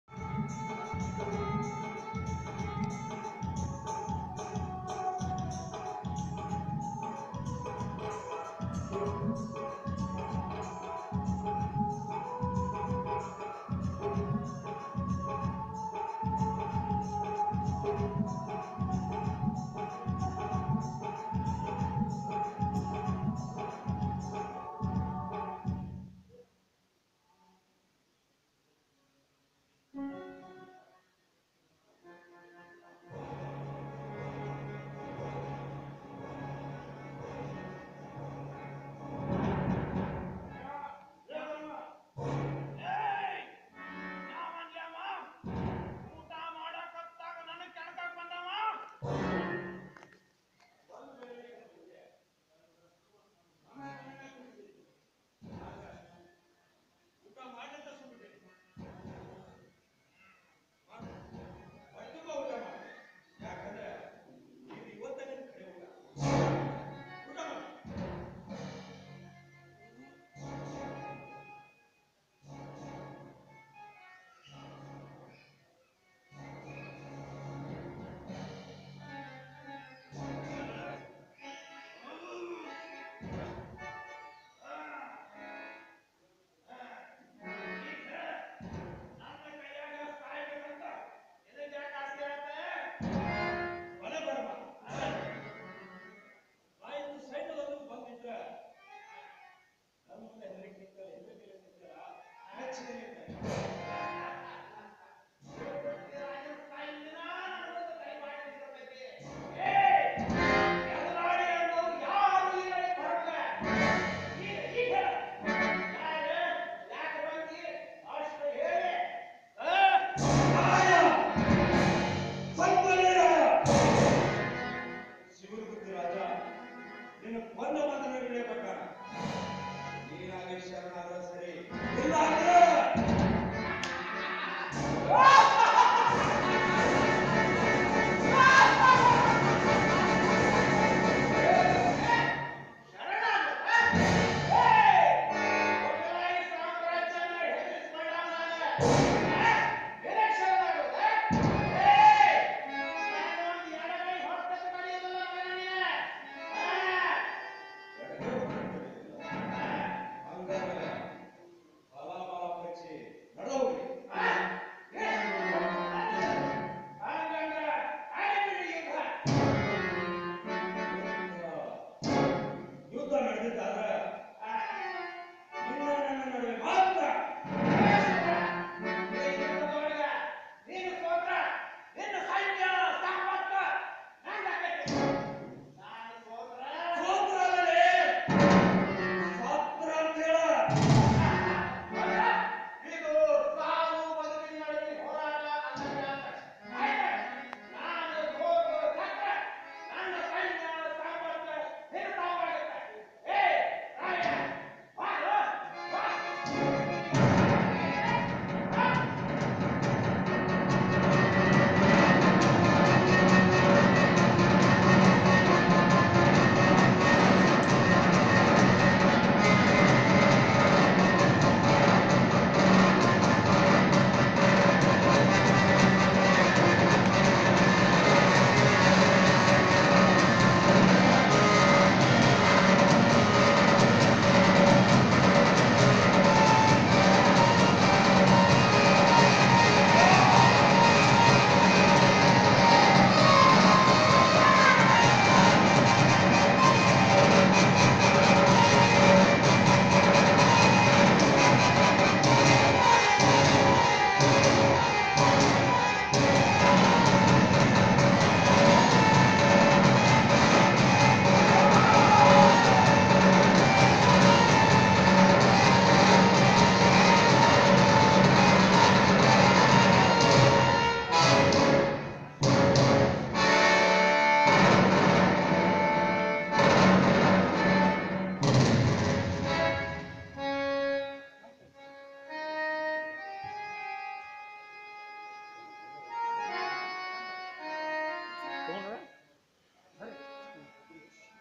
Badari as Bharama in historical drama Sahasaveer Sangolli Rayanna.
still from video of historical drama Sahasaveer Sangolli Rayanna played on 26/01/2019 Saturday in the memory of Republic day n Sangolli Rayanna's hanged day.